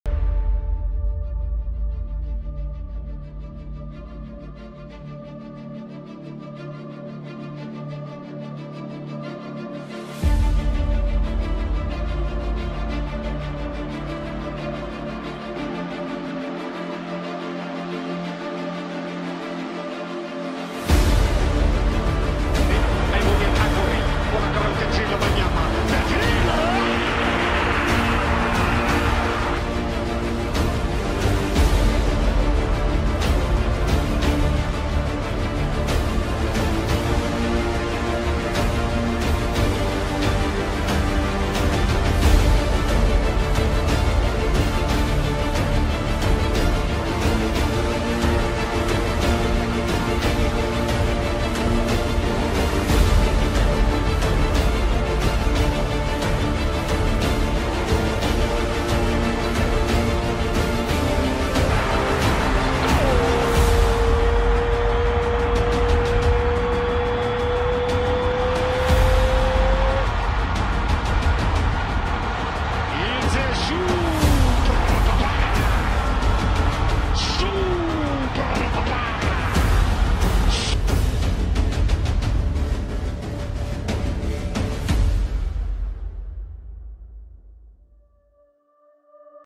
Kaizer Chiefs VS Orlando Pirates Trailer(06 November 2021)
Bringing you the first Soweto derby trailer for the new season .the hype is up this Saturday when low performing Kaizer Chiefs faces on song Orlando Pirates at the FNB this Saturday, for 3 points and BRAGGING RIGHTS.